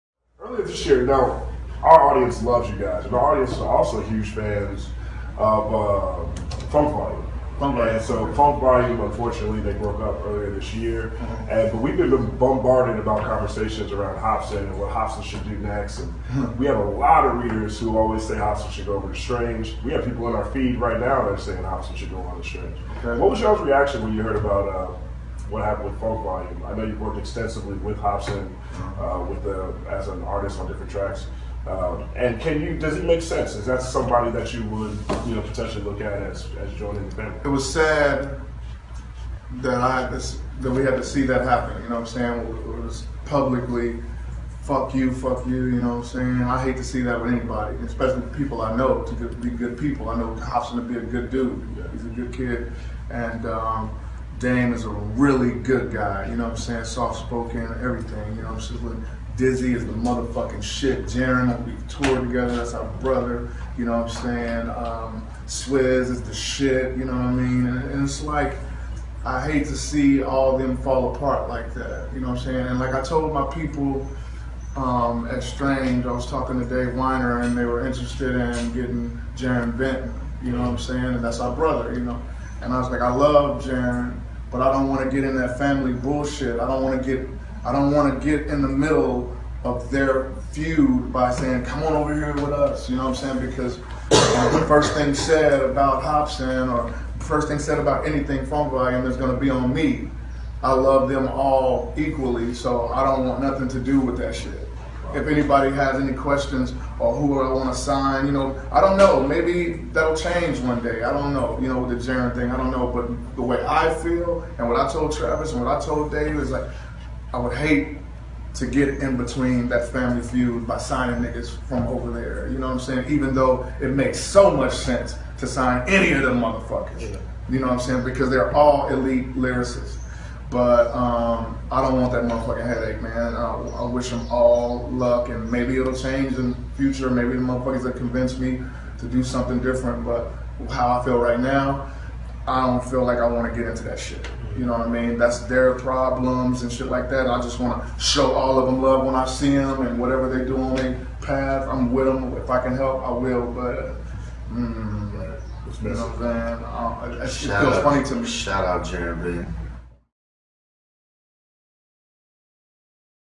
Here's Why Tech N9ne Won't Sign Hopsin
From our Facebook Live Stream featuring Tech N9ne, Krizz Kaliko, & Rittz from Strange Music.